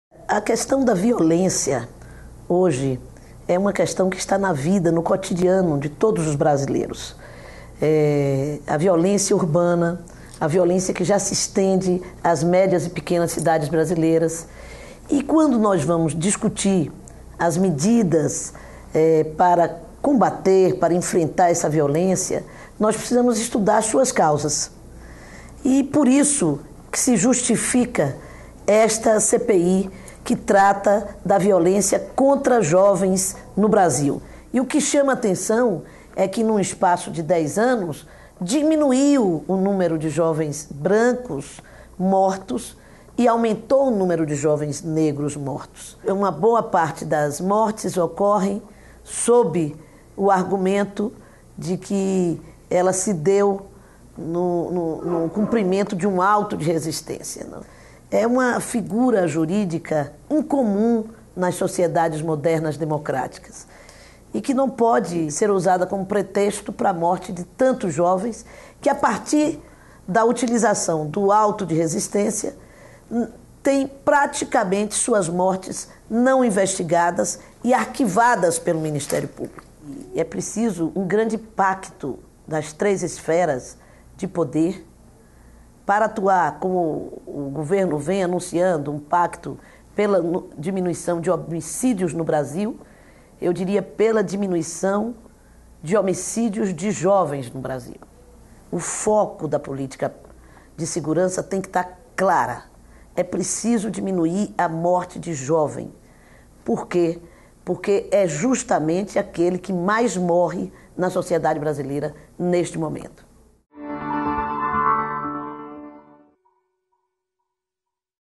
Lídice pede "pacto" para conter assassinato de jovens no Brasil
Presidente da CPI do Assassinato de Jovens, a senadora Lídice da Mata (PSB-BA) considera que é necessário um “grande pacto” no país pela diminuição dos homicídios de jovens. Desde a sua instalação, em maio, a comissão de inquérito ouviu dezenas de especialistas e parentes de vítimas. Entre as conclusões tiradas dos testemunhos, estão a existência de um componente racial nessa violência, que atinge sobretudo os negros, e o impacto da falta de investigação de muitas mortes ocorridas em operações policiais. A violência contra os jovens é o tema do Especial Cidadania, seção do Jornal do Senado, na edição da próxima terça-feira (8).